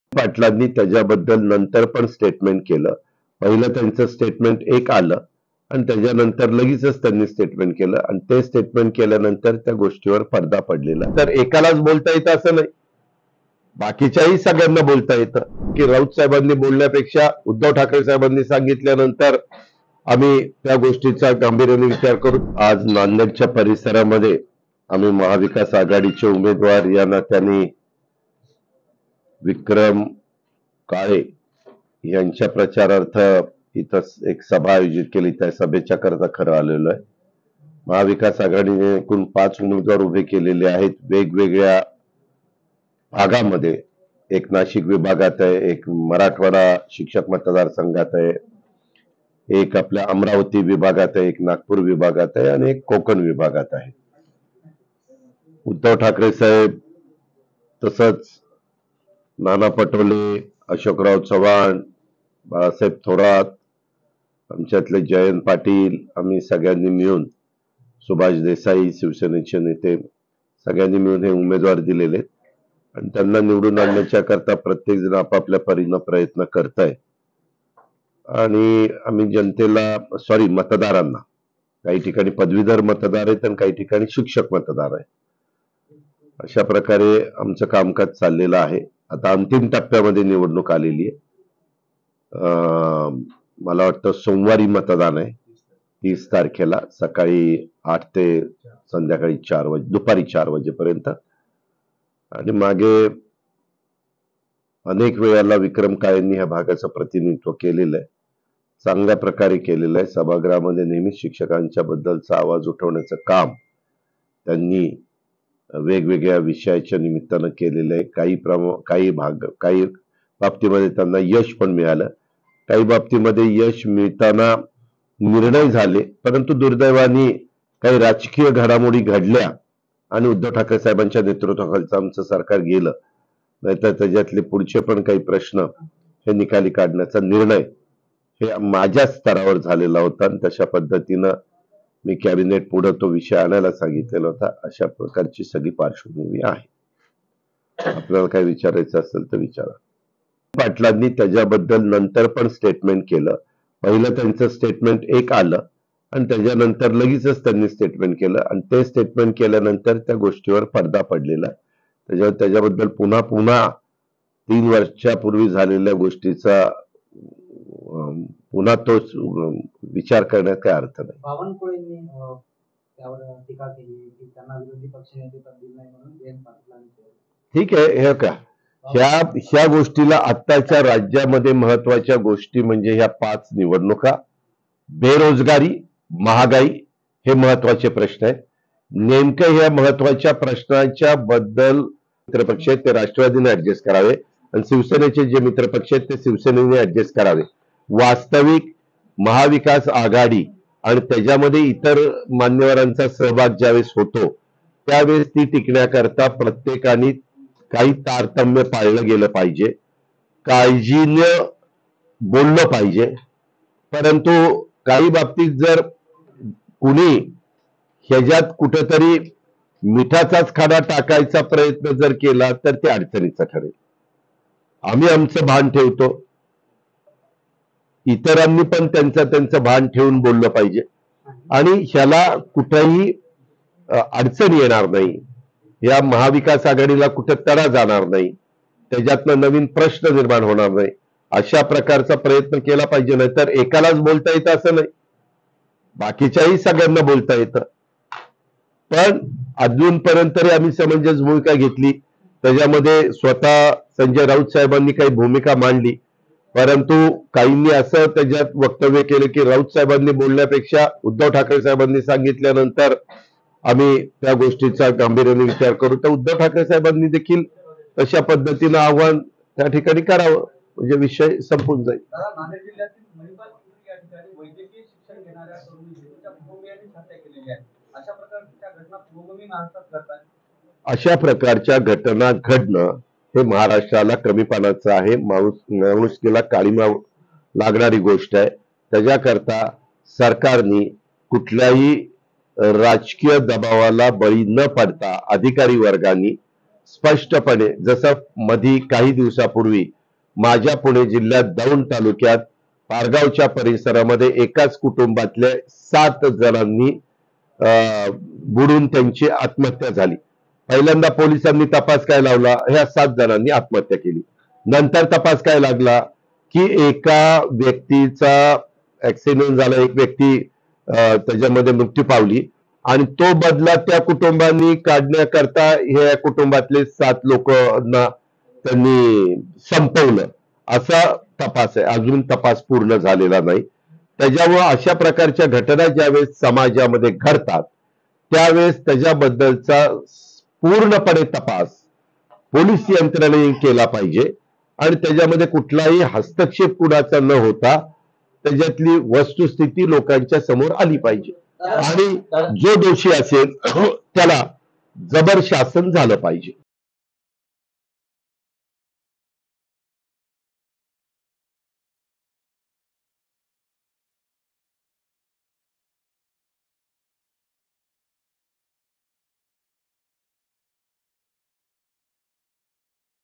Ajit Pawar : अजित पवारांचा सल्ला कुणाला? | LetsUpp Marathi
विधानसभा विरोधी पक्ष नेते अजित पवार हे नांदेड दौऱ्यावरती आले असता त्यांनी एमजीएम कॉलेज येथे मराठवाडा शिक्षक मतदारसंघाच्या प्रचारार्थ जाहीर सभा घेतली. त्यानंतर त्यांनी पत्रकार परिषदेत अनेक मुद्द्यांवर भाष्य केले.